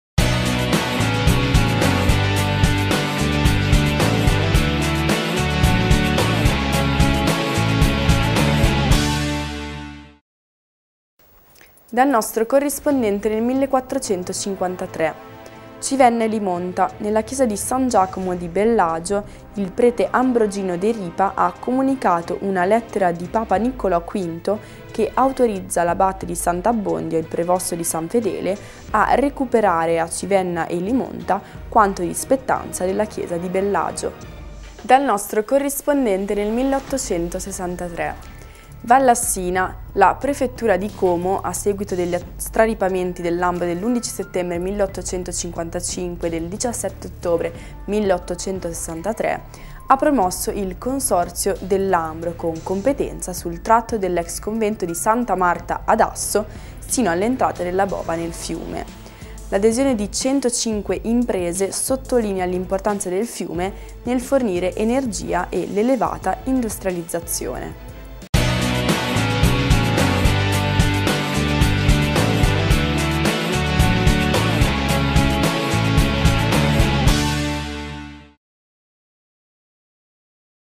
Accade in Vallassina - CIVENNA E LIMONTA, CIAO BELLAGIO
Accade in Vallassina - 15 Novembre

-) CIVENNA E LIMONTA, CIAO BELLAGIO
-) STOP AGLI STRARIPAMENTI

Ricerche, testi e redazione di Paolo Ceruti